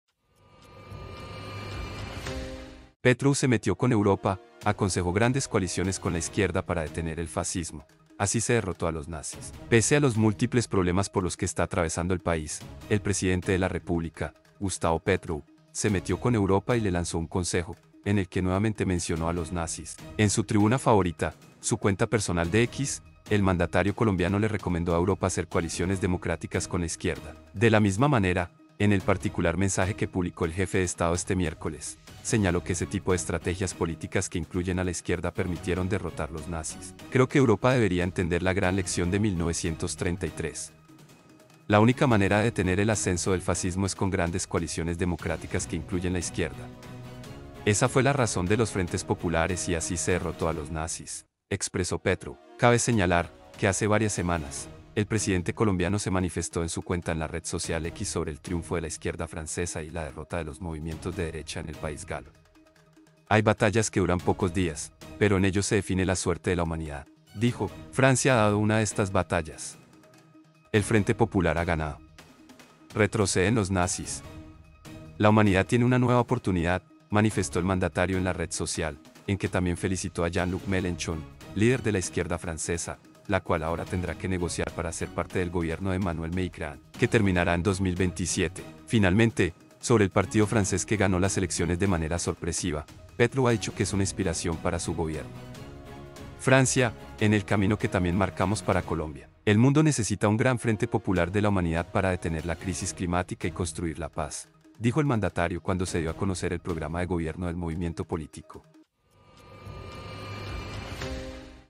ATENCIÓN ⛔ PETRO SE METE AHORA CON EUROPA
Pese a los múltiples problemas por los que está atravesando el país, el presidente de la República, Gustavo Petro, se metió con Europa y le lanzó un consejo.
SEMANA